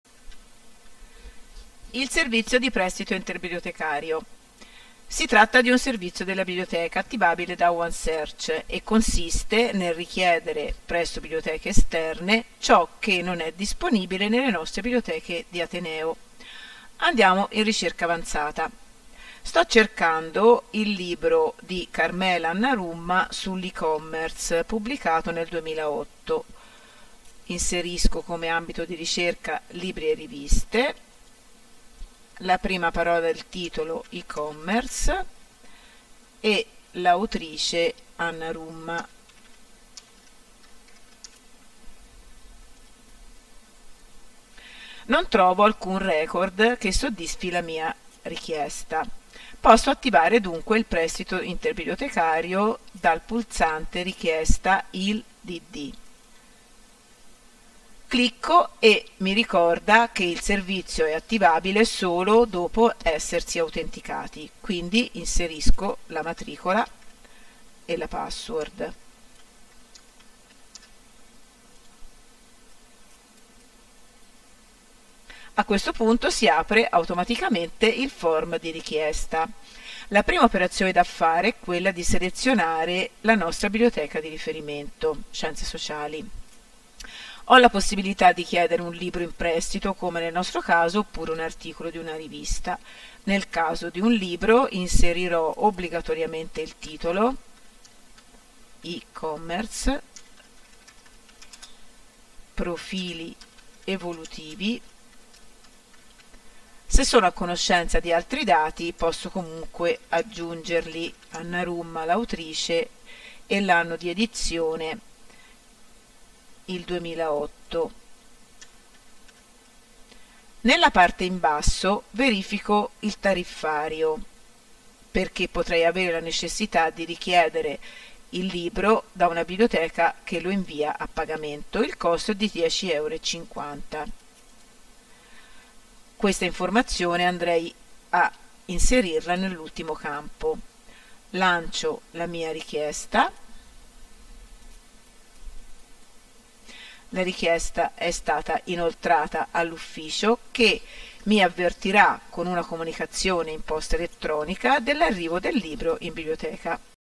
Come attivare il servizio ILL/DD
Tutorial sul servizio di prestito interbibliotecario e fornitura copie (ILL-DD): per richiedere libri in prestito, copie di articoli e di parti di libri non posseduti dalle biblioteche di Ateneo. Maggiori informazioni